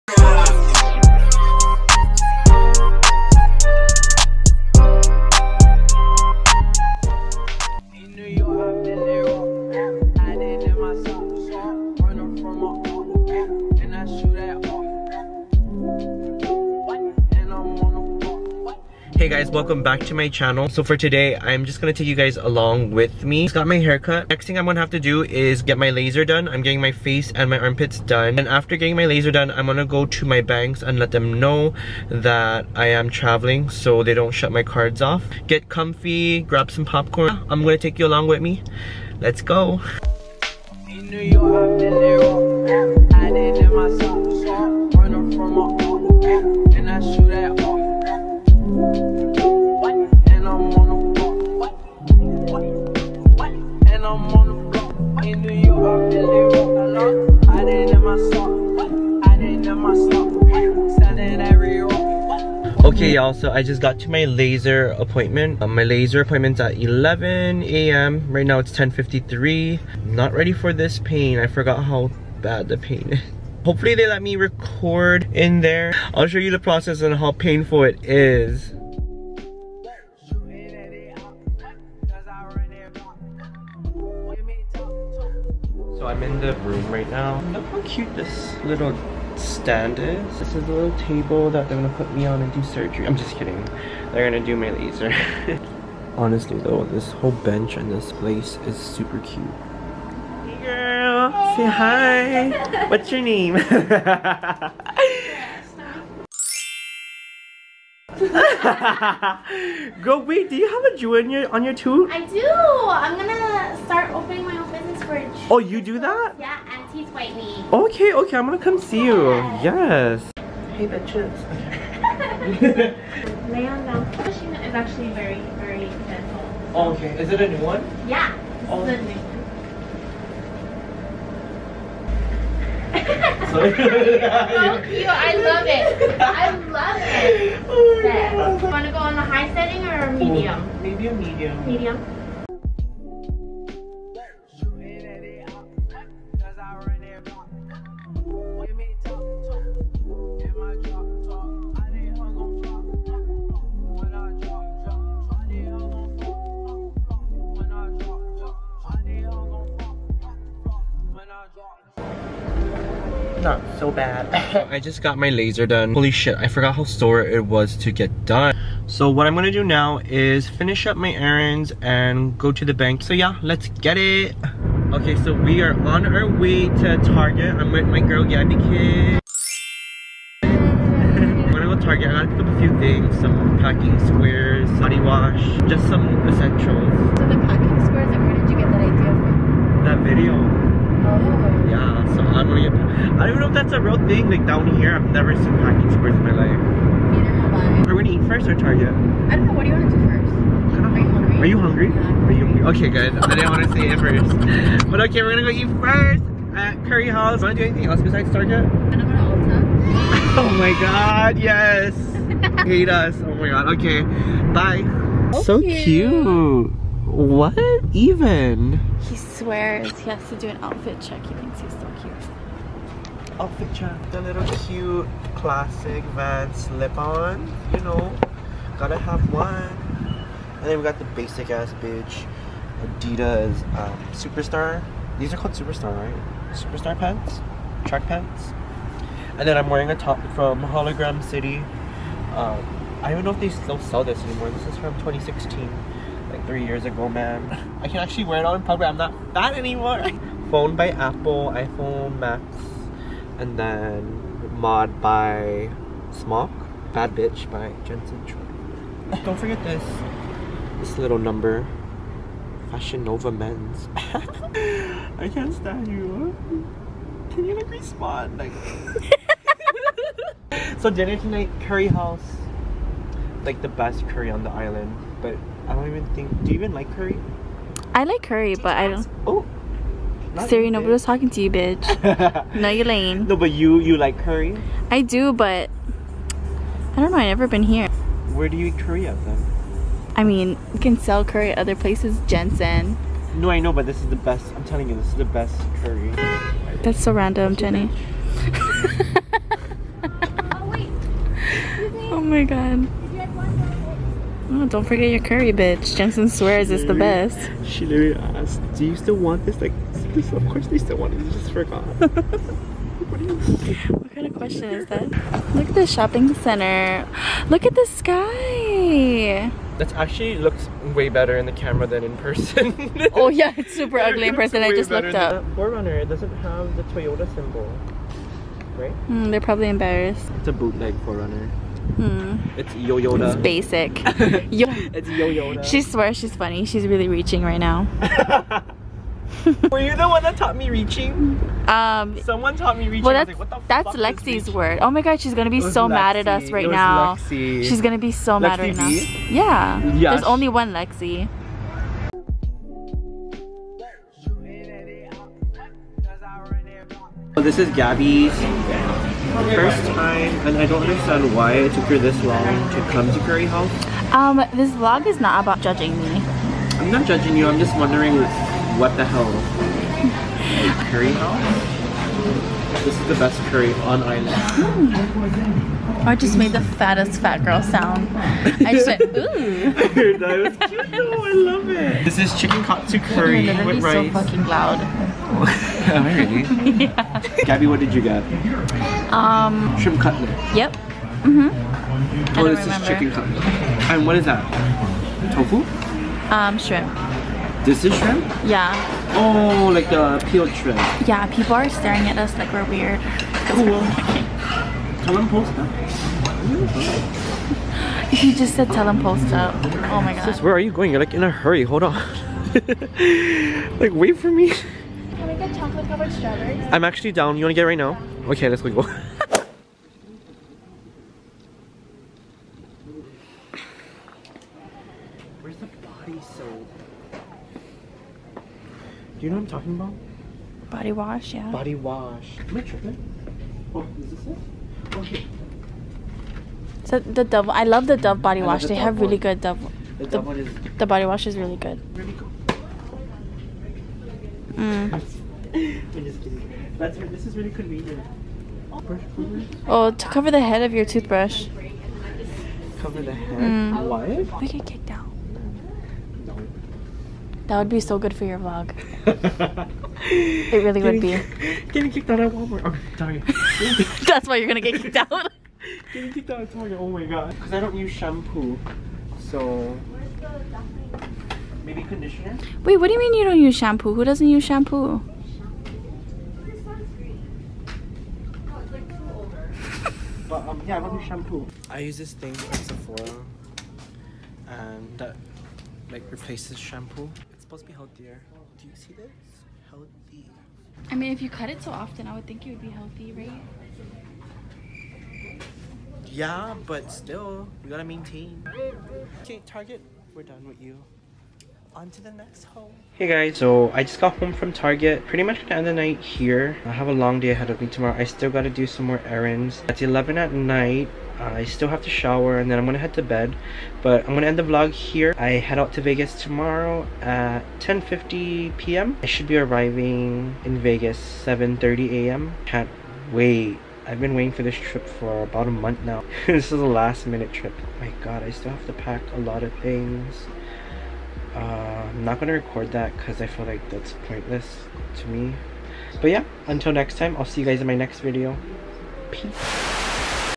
VEGAS TRIP PREP
hey y’all, welcome back to channel!!! today i’m bringing y’all along with with me to LA LaserHI, Curry House, & Target. Y’all also get meet my girlllll Gabby🥰💕

🔖Book a laser appointment with Deedra!
Follow my girl Gabby!
sc: gabbbykins_